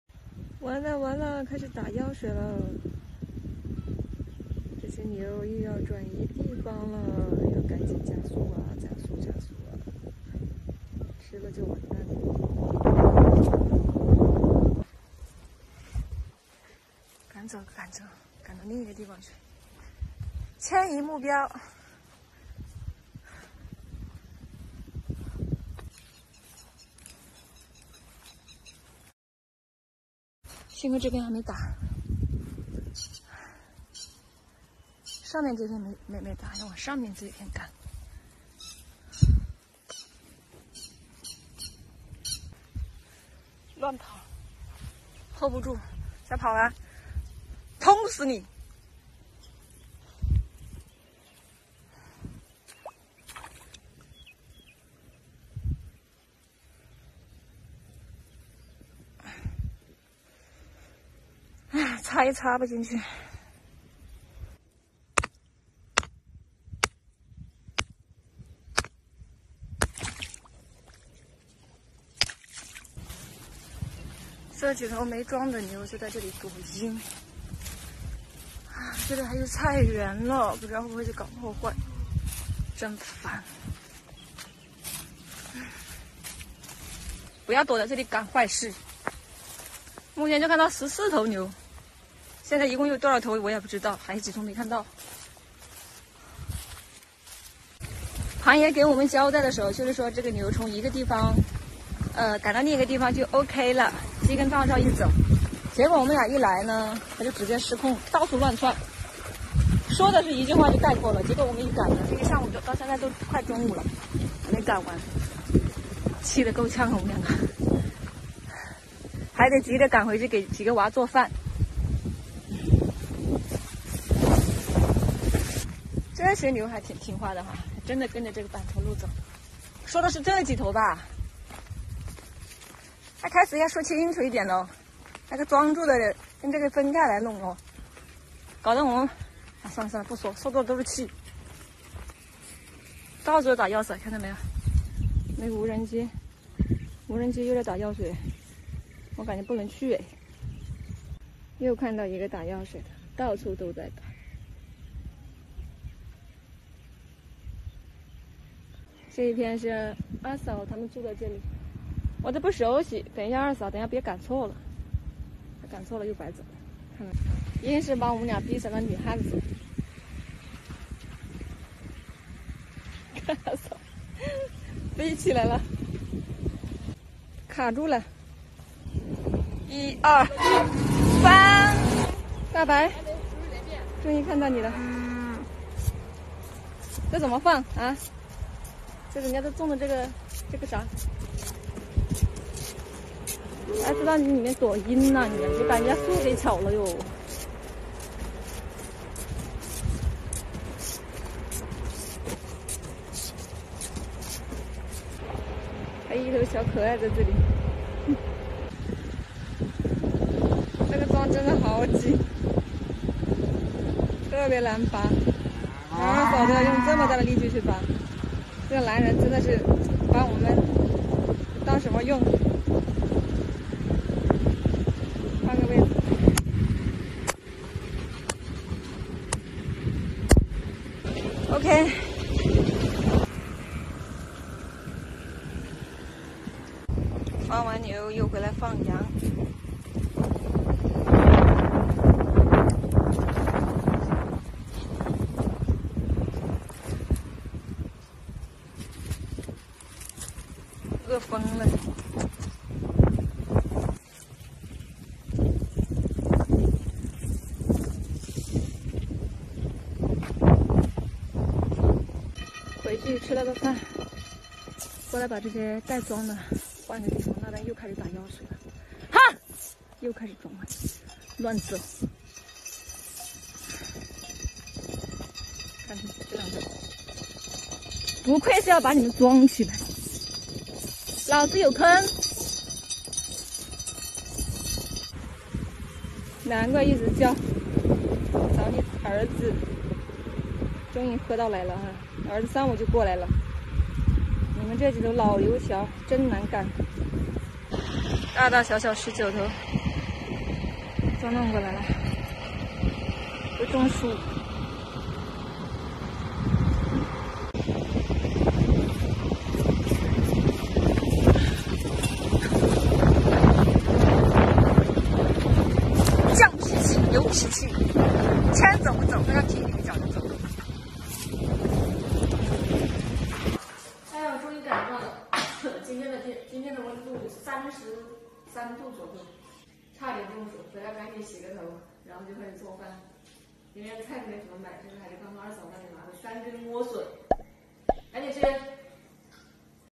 田里打除草剂了，两个牛媳妇顶着33°高温把19头牛换个位置，哭了
1

大家好！
这里是【恋上小虎牙】YouTube唯一官方频道
我是螃爷的老婆
大家可以叫我螃嫂哦~